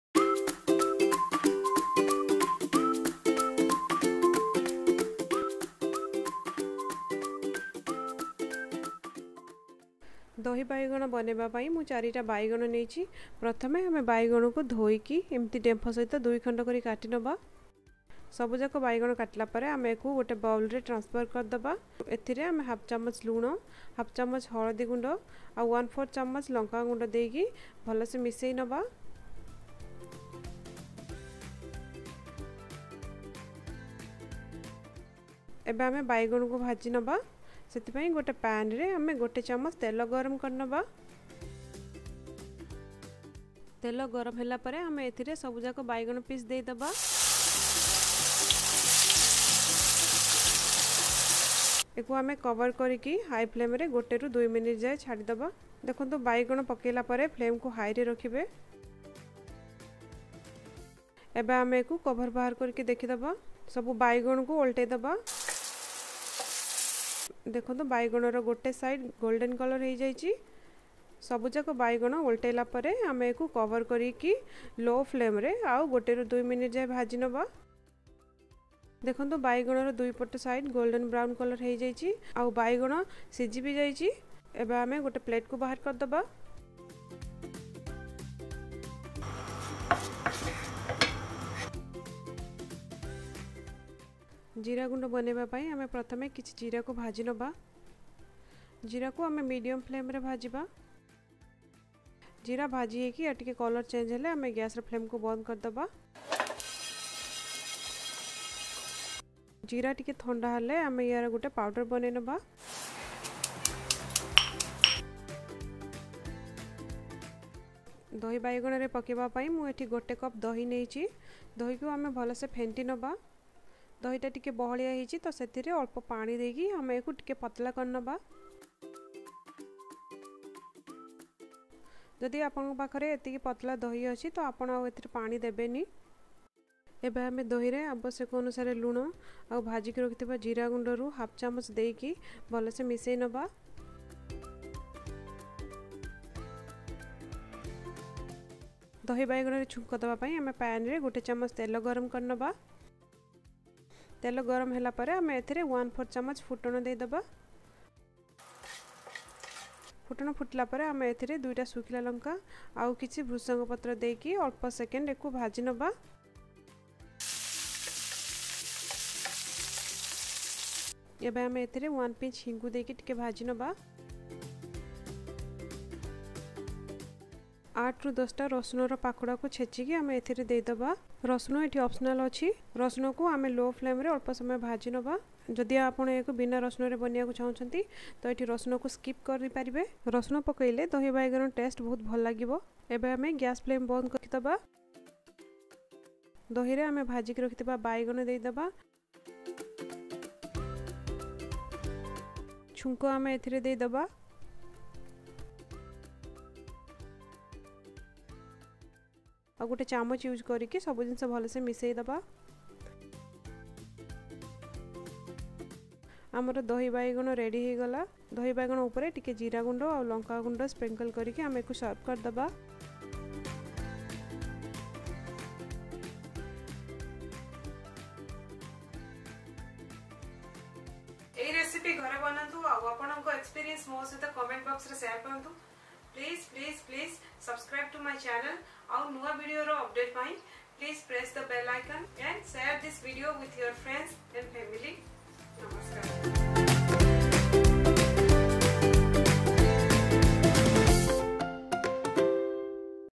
ଦହି ବାଇଗଣ | Dahi Baigana Recipe in Odia | Dahi Baingan | Curd Brinjal Recipe | Eli's Vlog Odia
Ingredients:
Brinjal: 4 Nos
Turmeric Powder: 1/2 tsp
Red Chili Powder: 1/4 tsp
Oil
Curd: 1 Cup
Roasted Cumin Powder: 1/2 tsp
Panch Phoron: 1/4 tsp
Dry Red Chilies: 2 Nos
Curry Leaves: 7-8 Nos
Hing/Asafoetida: 1 Pinch
Garlic Cloves: 8-10 Nos(Optional)